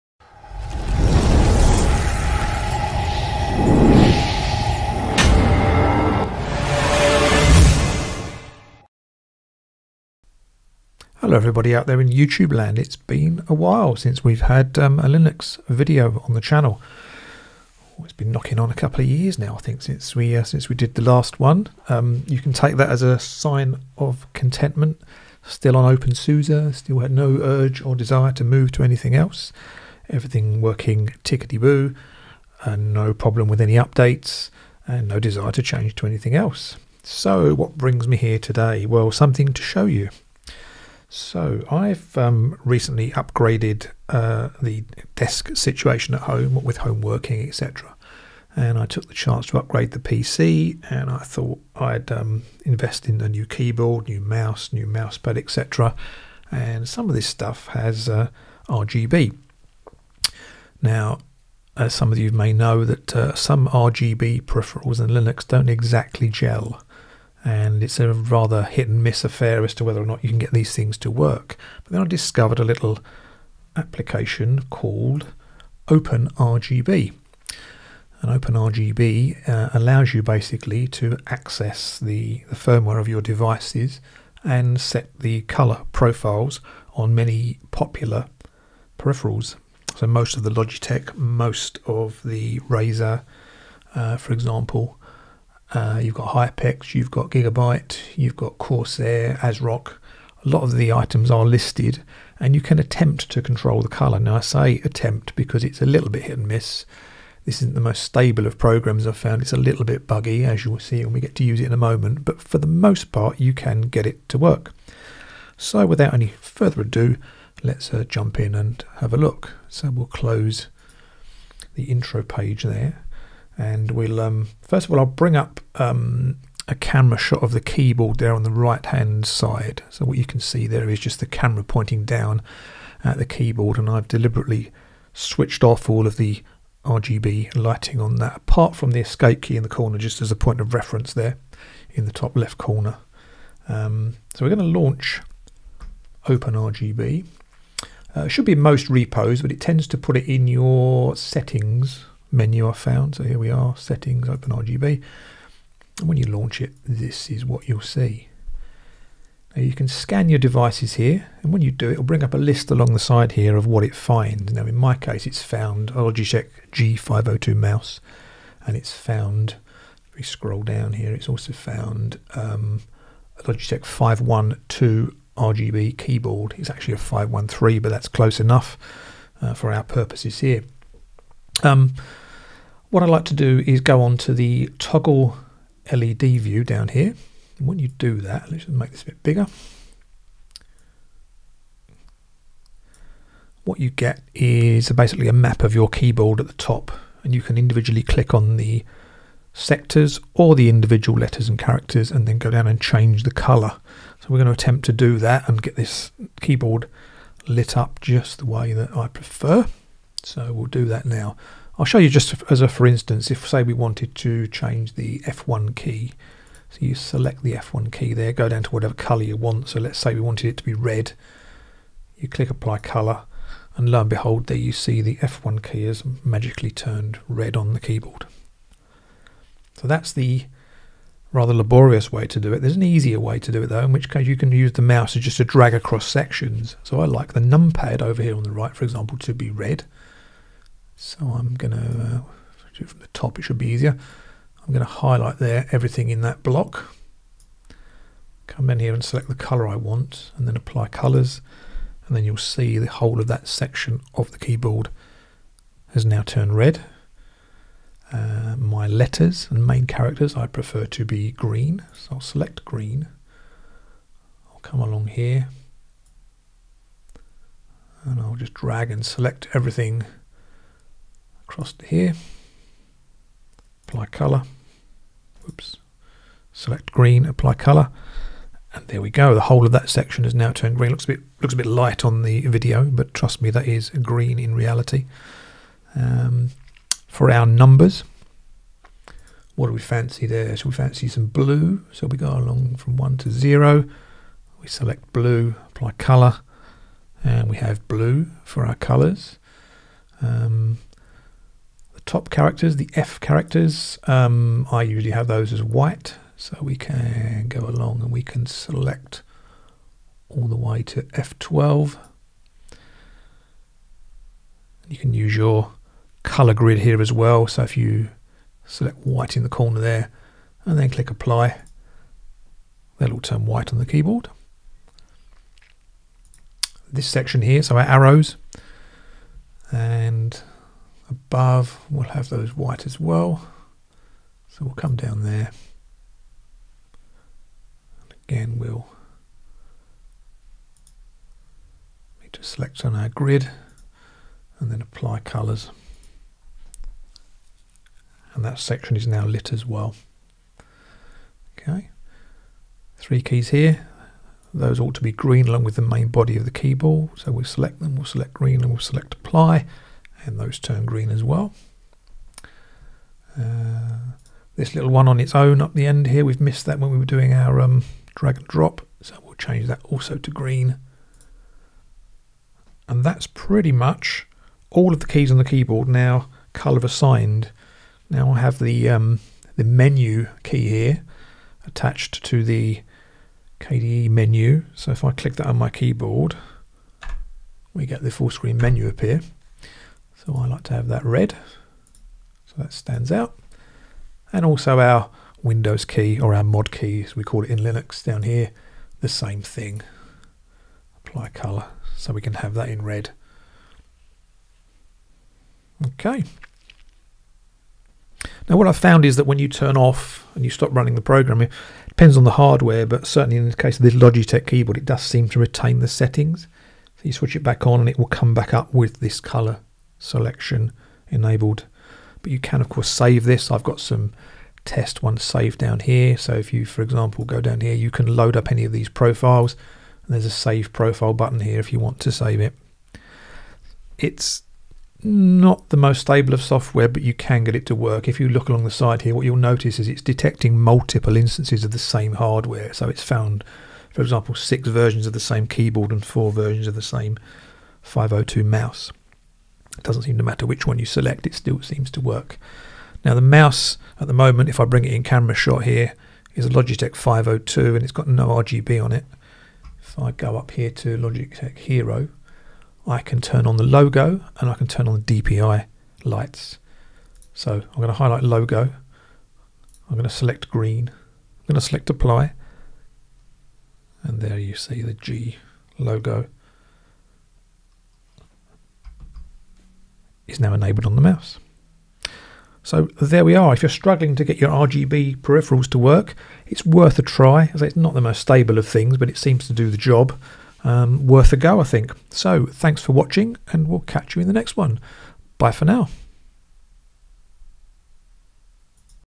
OpenRGB - Control RGB lighting on Linux
Most RGB peripherals have Windows or Mac only control software. With OpenRGB you can control your RGB peripheral lighting on Linux.